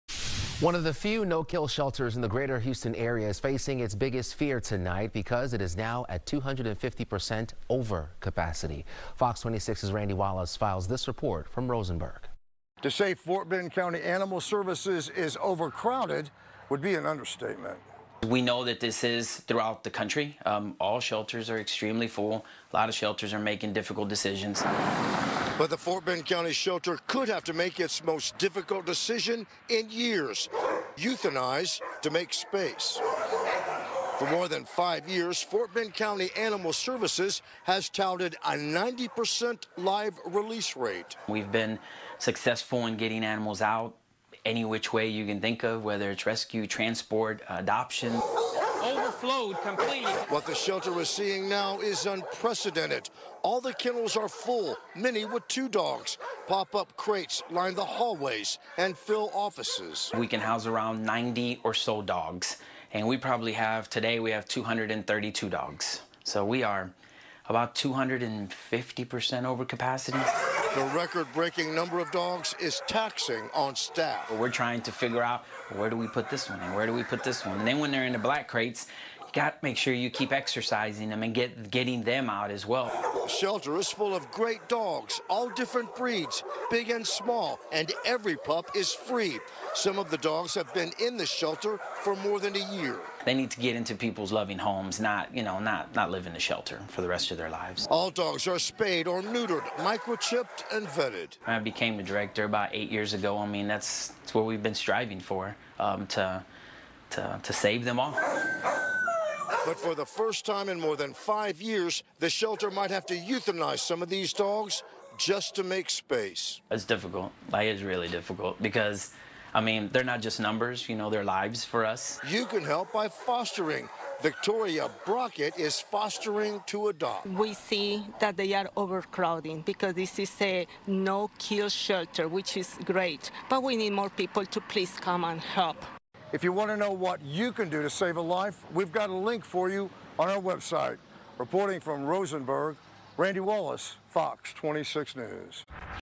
Fort Bend County animal shelter overcrowded, 250 percent over capacity
One of the few no-kill shelters in the Greater Houston area is facing it's biggest fear because it's 250 percent over capacity. FOX 26's Randy Wallace has the report.